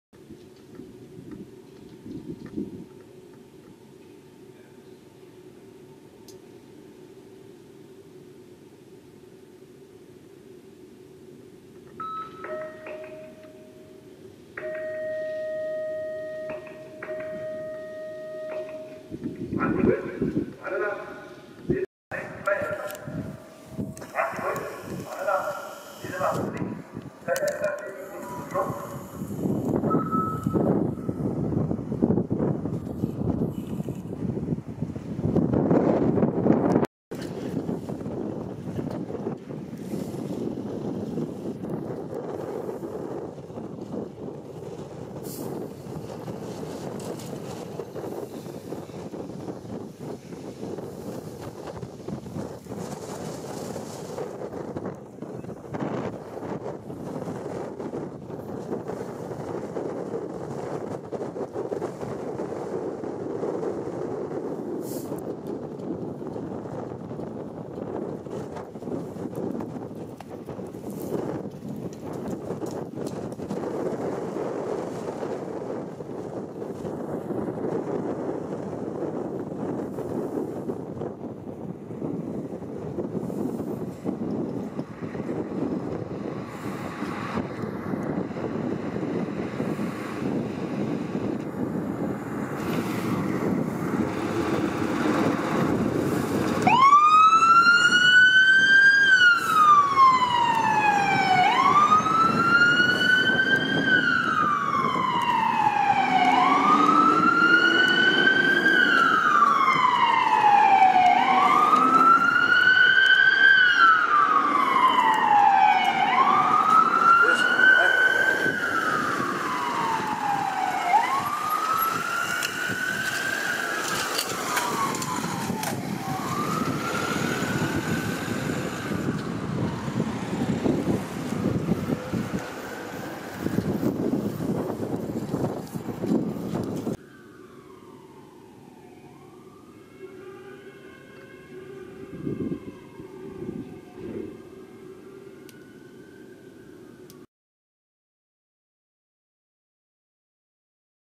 hovedstadens beredskab ST.FB TLF R19 brandbil i udrykning Feuerwehr auf Einsatzfahrt
sprøjte r19 fra st.frederiksberg kører til automatisk brandalarm på lindevangs alle på frederiksberg københavn

pump r19 from st.frederiksberg drive to automatic fire alarm on lindevangs alle on frederiksberg copenhagen